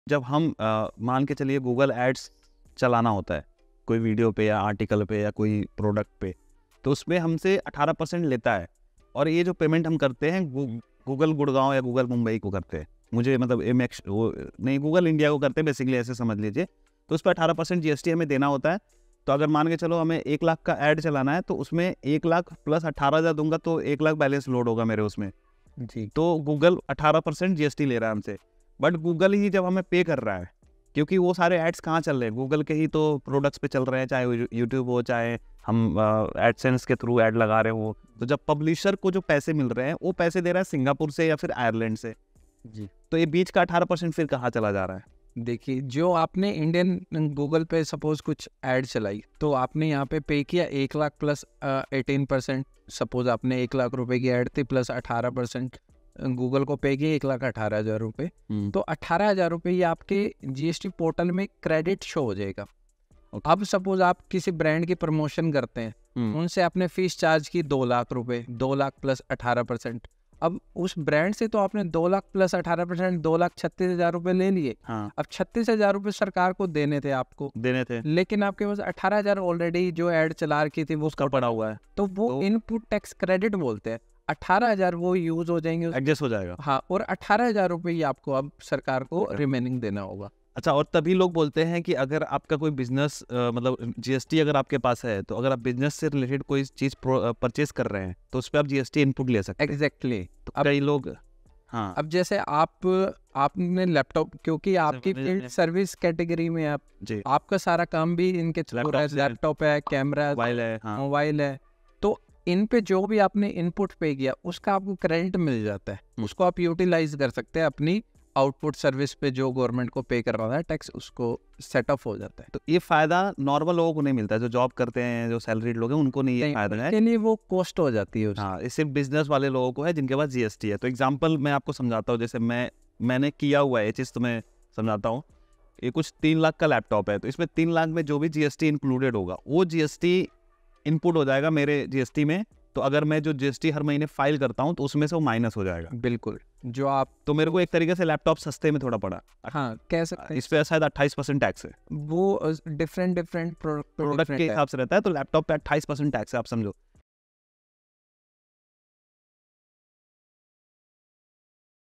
Google Ads से Tax कैसे बचाये? | Tax Credit on Google Ads Spend
(Send us your invoice and get $1000 worth of premium plugins and themes for free)

Namaste Dosto,
In this video, CA, Mr. Ashwani Arora is sharing tips on How to get GST Tax credit on Google Ads spend? you can also watch full video on Satish K Videos main channel